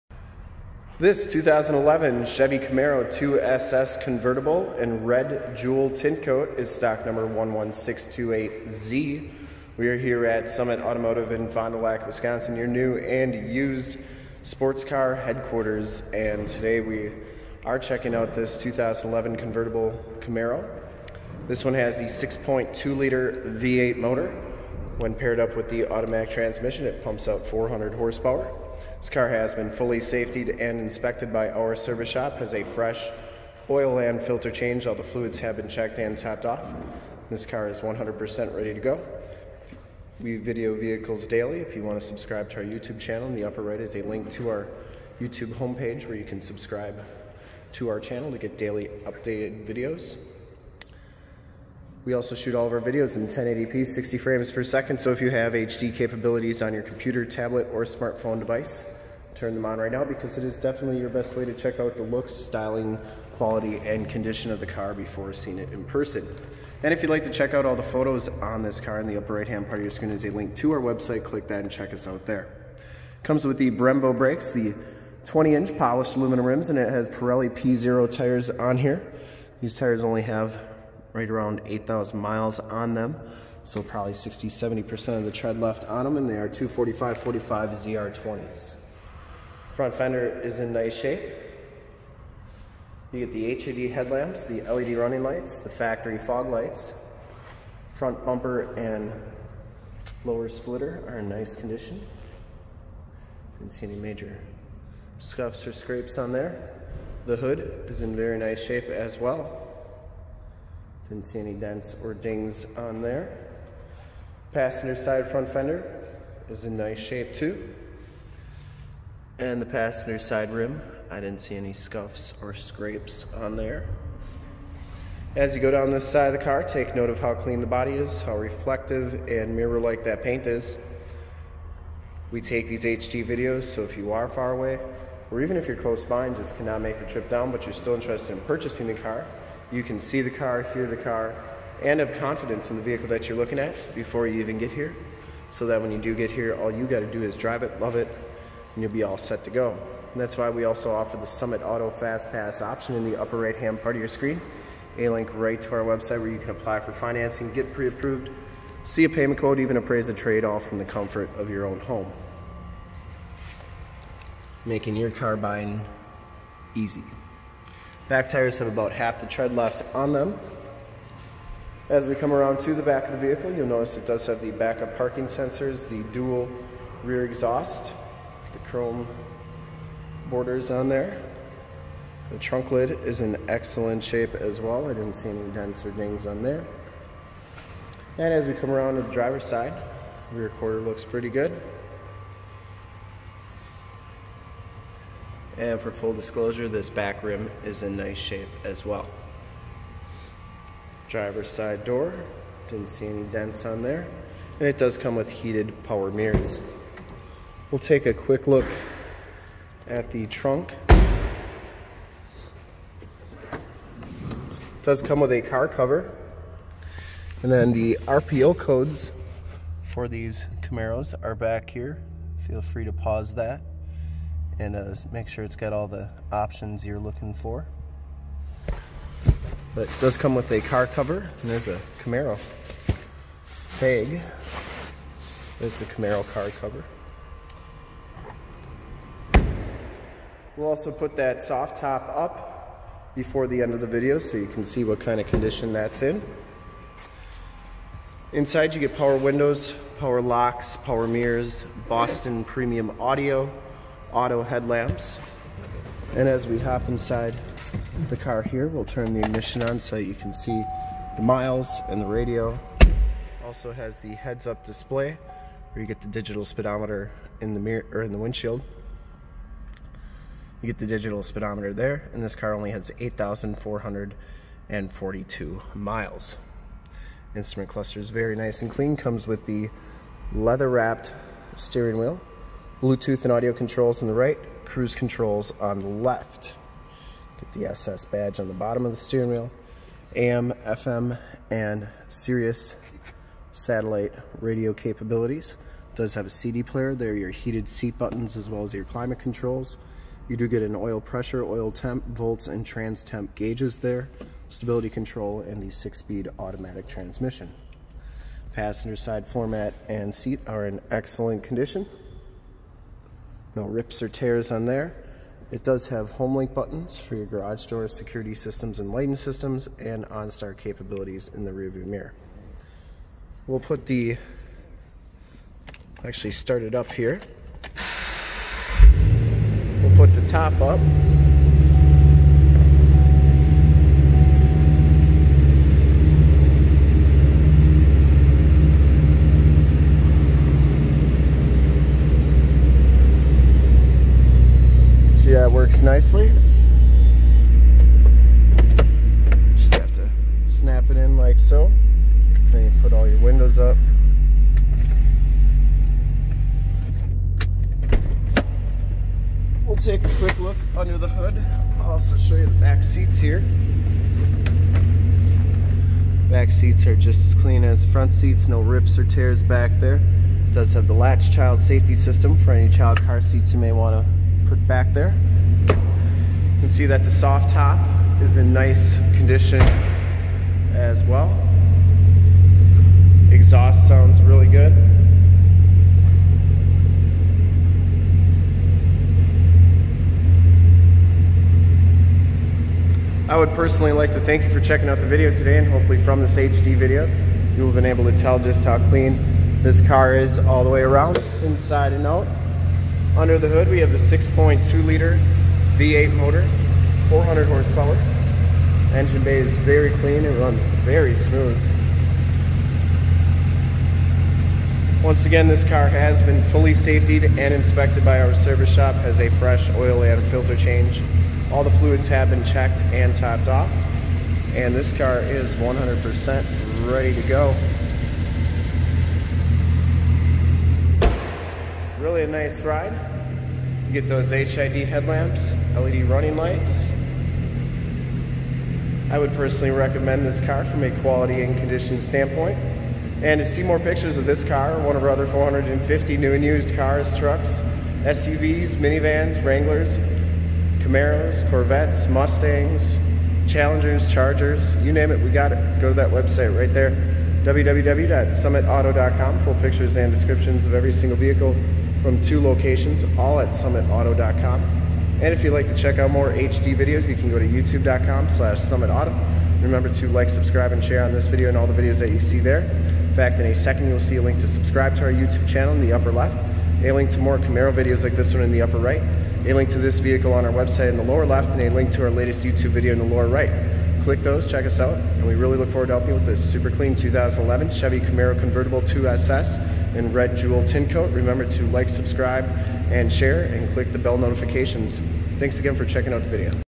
2011 CHEVROLET CAMARO 2SS CONVERTIBLE AUTOMATIC RED JEWEL TINCOAT WALK AROUND REVIEW 11628Z SOLD!
This 2011 CHEVROLET  CAMARO 2SS CONVERTIBLE AUTOMATIC IN RED JEWEL TINTCOAT FOR SALE IN FOND DU LAC OSHKOSH WISCONSIN 54935 is the vehicle we did walk around review of today.

Thank you for checking out this video of this CHEVROLET  CAMARO 2SS CONVERTIBLE AUTOMATIC IN RED JEWEL TINTCOAT FOR SALE IN FOND DU LAC OSHKOSH WISCONSIN 54935

STOCK: 11628Z
PRICE: $28,999
MILES: 8,411
MAKE: CHEVROLET
MODEL: CAMARO
VIN: 2G1FK3DJ1B9181054
LOCATION: FOND DU LAC OSHKOSH WISCONSIN, 54937 TRUCKS ON 41

1 OWNER! CLEAN TITLE HISTORY! 6.2 Liter OHV V8 Engine, 400 Horsepower, Two Door Convertible, 2SS Package SS 2, 6 Speed Automatic Transmission, Automatic Transmission Center Console Shifter, Rear Wheel Drive RWD 4x2 Two Wheel Drive, Dual Rear Exhaust, Onstar System, Power Driver's Seat, Dual Heated Seats, Non Smoker, Black Ebony Leather Seats, Bucket Seats, Heated Power Mirrors, Stabilitrak Traction Control, Pirelli Pzero P275/40 ZR20 Tires, 20 Inch Rims Premium Wheels, Polished Aluminum Rims Premium Wheels, Four Wheel Disc Brembo Brakes, Decklid Spoiler, Fog Lights, HID High Intensity Discharge Headlights, LED Running Lights, Reverse Sensors, Chrome Tipped Exhaust, AM / FM Radio Tuner, Sirius/XM Satellite Radio Capabilities Sirius / XM, CD Player, Boston Premium Audio Sound System, Bluetooth, Hands-Free Phone Controls Blue Tooth, Auxiliary MP3 Jack Portable Audio Connection, USB Jack Portable Audio Connection, Keyless Entry with Factory Remote Start, Rear Window Defroster, Driver and Passenger Front Air Bags, L.A.T.C.H. Child Safety System, Side Curtain Air Bags SRS Safety Restraint System, Multi-Function Steering Wheel Controls, Homelink System with Three Programmable Buttons for Garage Doors, Lighting Systems & Security Systems, Heads up Display, Factory Floormats, Air Conditioning AC, Cruise Control, Power Locks, Power Windows, Automatic Headlights Autolamp, Tilt/Telescope Steering Wheel, Red Jewel Tintcoat Maroon, ONE OWNER! CLEAN AUTOCHECK! Very very clean inside and out! This is one of the sharpest 2011 Chevrolet Camaro convertable cars we have ever had on our lot! Make your move before this super clean ride is gone!

STOCK: 11628Z
PRICE: $28,999
MILES: 8,411
MAKE: CHEVROLET
MODEL: CAMARO
VIN: 2G1FK3DJ1B9181054